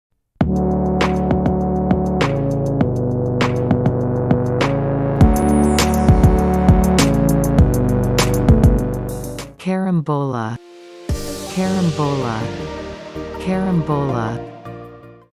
Correct Pronunciation Of Carambola | 2020
Watch the video if you want to learn correct way to pronounce the word